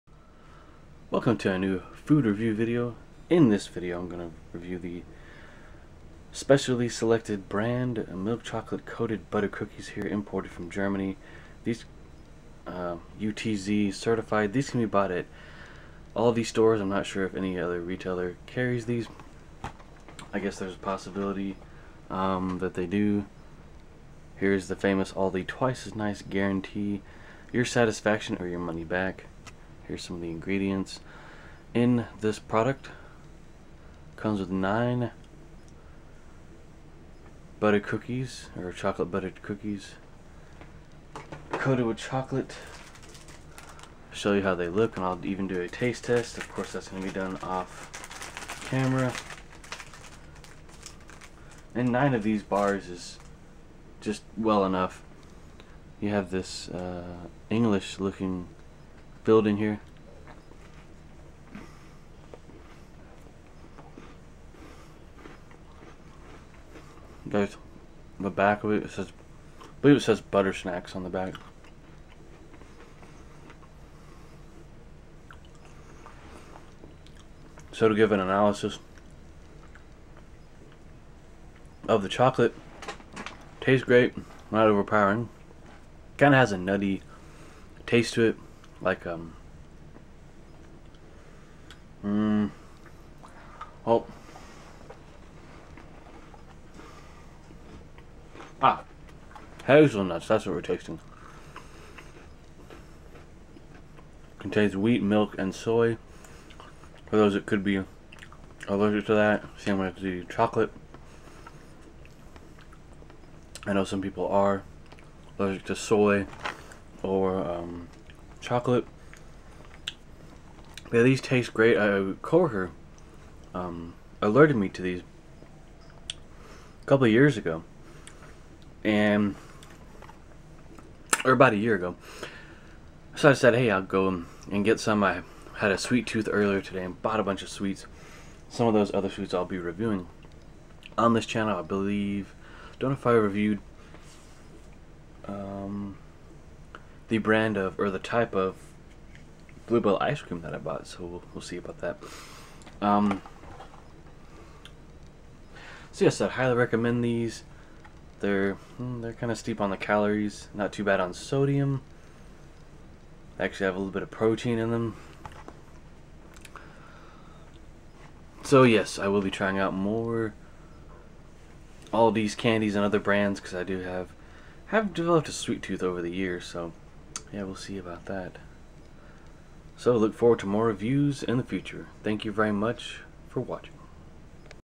Specially Selected (Aldi) Milk Chocolate Butter Cookie Review
These cookie snacks can be found in Aldi grocery stores. A co-worker told me about these a year ago and i tried them out and like them. Got around to buying another box of these and took that opportunity to give them a review.